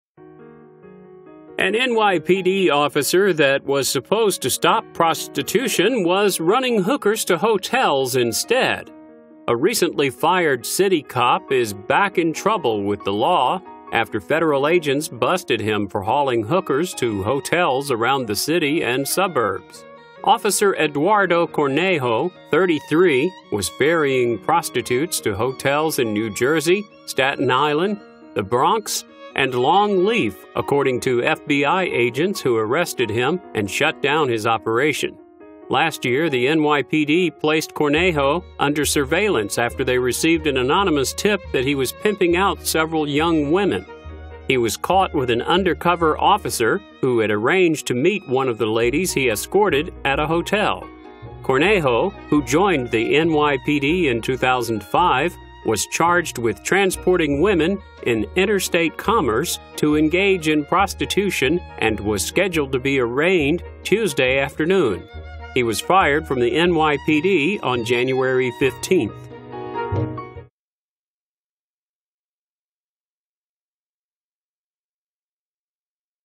BUSTED Former NYPD Cop Drove Prostitutes to Hotels
Officer Eduardo Cornejo was ferrying prostitutes to hotels